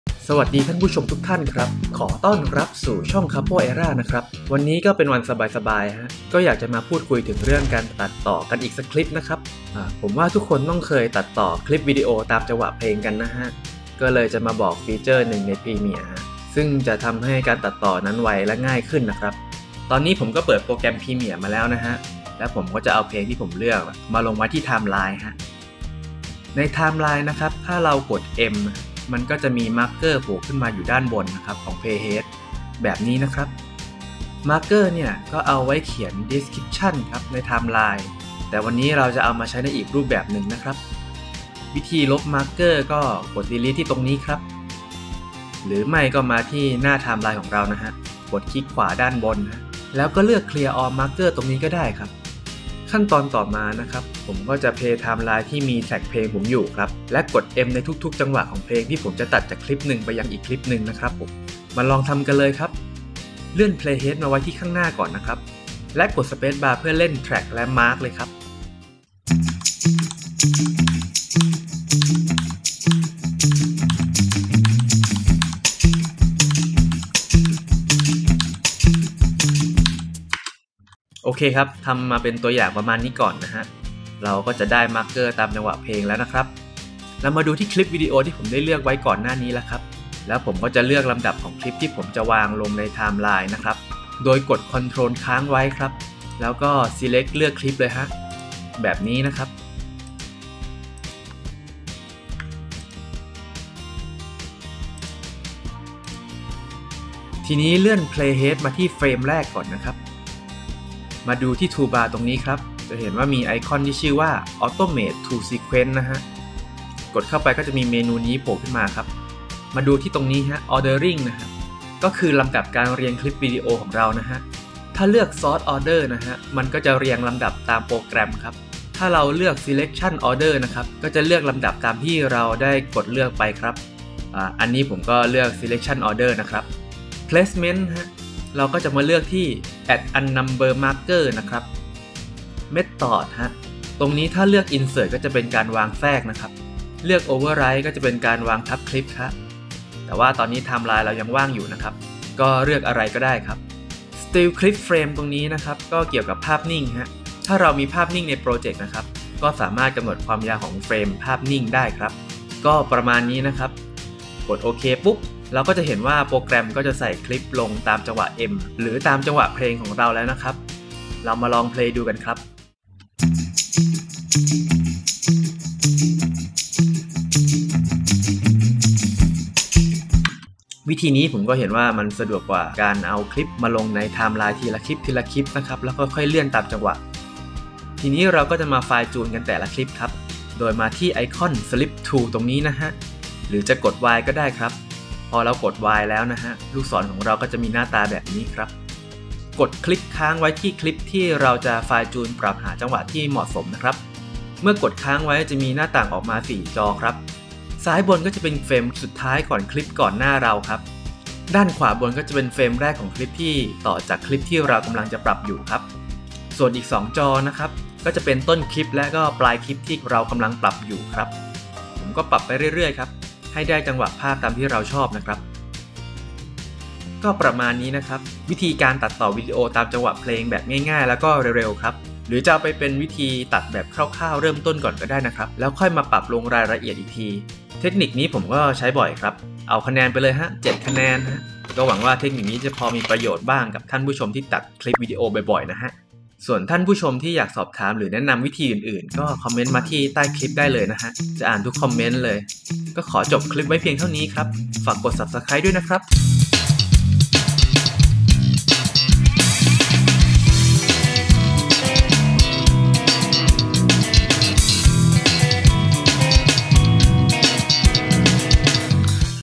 [ EditorTrick ] ตัดต่อตามจังหวะเพลงอย่างรวดเร็ว
Youtuber ทุกคนต้องเคยตัดต่อวีดีโอตามจังหวะเพลงใช่ไหมครับ
ก็เลยจะมาพูดถึง ฟีเจอร์หนึ่งใน Premiere cc2019 ซึ่งอาจจะทำให้ให้การตัดต่อนั้นไวและง่ายขึ้นครับ

[ EditorTrick ] Multi-Camera in Premiere Pro ตัดต่อได้ว่องไวในหลายมุม !!

[ PhotoshopTrick ] Super resolution ขยายภาพด้วย file .RAW .JPG โดย Adobe Photoshop 2021